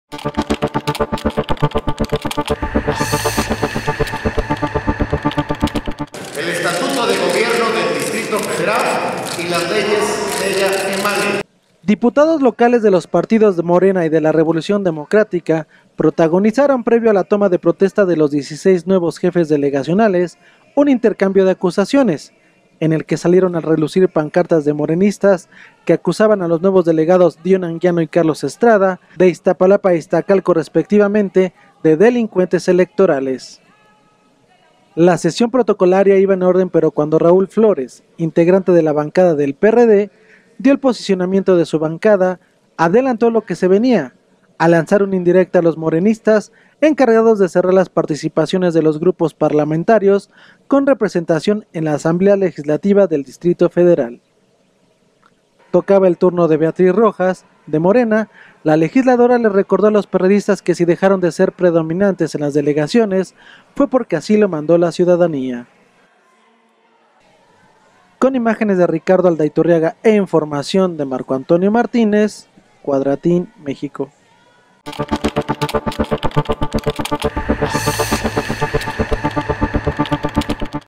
Acusa Morena a Dione Anguiano y Carlos Estrada de delincuentes electorales
MÉXICO, DF, 1 de octubre de 2015.- Diputados locales de los partidos Morena y de la Revolución Democrática (PRD) protagonizaron previo a la toma de protesta de los 16 nuevos jefes delegacionales, un intercambio de acusaciones, en el que salieron a relucir pancartas de morenistas que acusaban a los nuevos delegados Dione Anguiano y Carlos Estrada, de Iztapalapa e Iztacalco, respectivamente, de delincuentes electorales.

La sesión protocolaria iba en orden pero cuando Raúl Flores, integrante de la bancada del PRD dio el posicionamiento de su bancada, adelantó lo que se venía, al lanzar una indirecta a los morenistas, encargados de cerrar las participaciones de los grupos parlamentarios con representación en la ALDF.